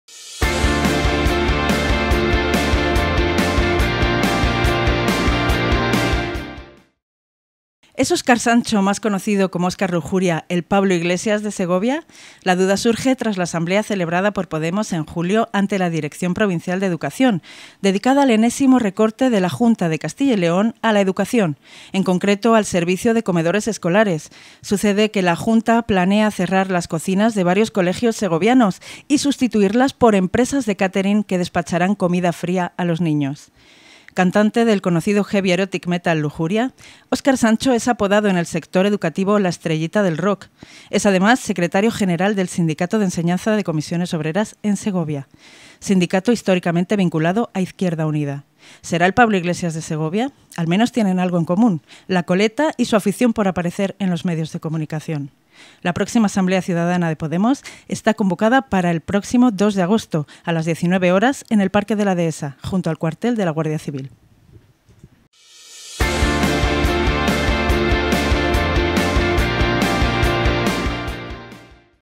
'Coleta star' en Podemos: ¿Es Óscar Lujuria el Pablo Iglesias de Segovia?
¿Es Óscar Sancho, más conocido como Óscar Lujuria, el Pablo Iglesias de Segovia? La duda surge tras la asamblea celebrada por Podemos el pasado mes de julio ante la Dirección Provincial de Educación, dedicada al enésimo recorte de la Junta de Castilla y León a la educación, en concreto al servicio de comedores escolares. Sucede que la Junta planea cerrar las cocinas de varios colegios segovianos y sustituirlas por empresas de catering que despacharán comida fría a los niños.